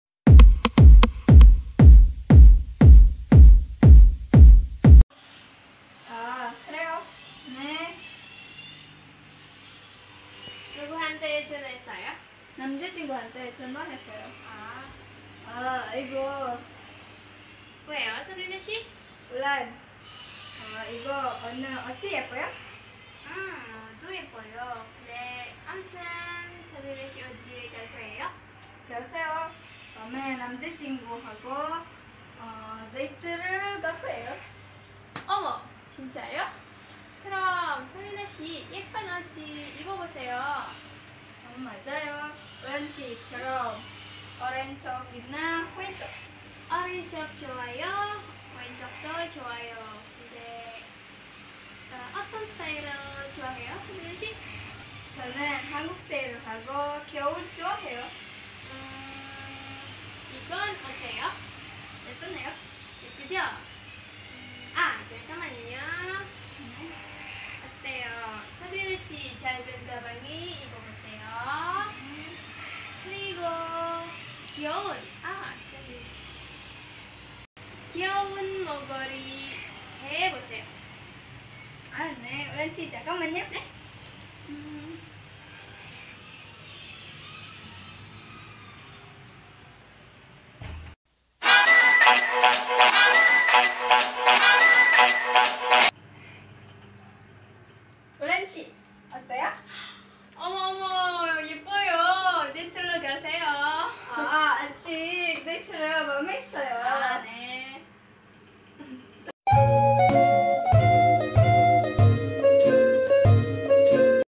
말하기 R 2 그룹 = 14 이름 = 울란, 사브리나 14 과, 숙제
안녕하세요...
우리는 우나스 대학생 입니다 🇮🇩
한국어를 공부합니다 🇰🇷🤗

Hallo Chingu!
Kami adalah mahasiswa Universitas Nasional (Unas) dengan jurusan bahasa korea... di video tsb kita belajar mengenai penggunaan kata :
1. 한테                 =  Kepada (Noun)
2. 아/어 보세요  =  Cobalah (Verb)
3. (은)ㄴ              =  Yang (Adjective)

Happy Watching
xoxo~